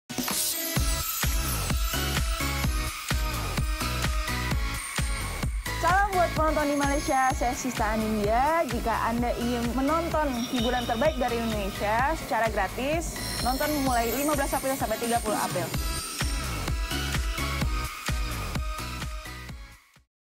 Jom Tonton Indopek secara Percuma!! (by Sistha)
Tonton secara percuma mulai 15-30 April 2016. Pelbagai program terbaik, terfenomena dan terkini dari Indonesia. Dua saluran dalam satu pakej yang hebat!! Hiburan Indonesia Tanpa Henti hanya di INDOPEK!! Maklumat lanjut di instagram @indopekastro atau Fb/indopek.